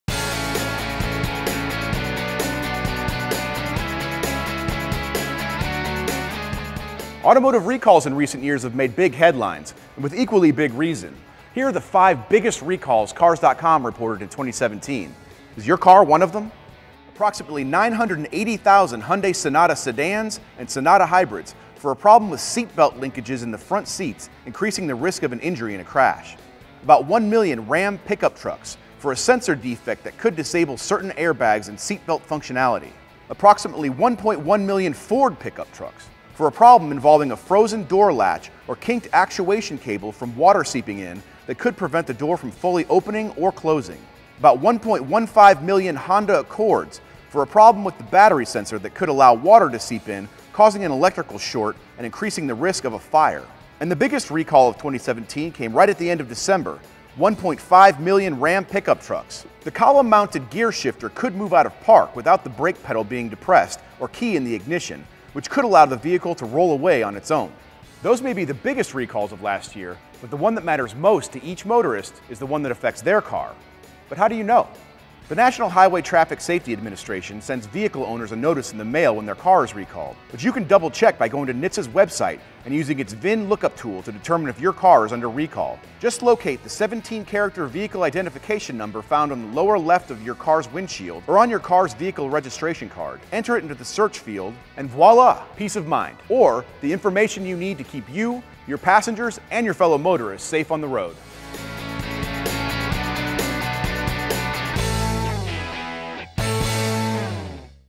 The Biggest Recalls of 2017 - Was Your Car One of Them?
In 2017 nearly 30 million vehicles were recalled for various safety reasons, was your car one of them? Matt Schmitz of Cars.com goes over some of the biggest recalls of the past year and how you can find out if your car is affected in this week’s segment of Driving Smart.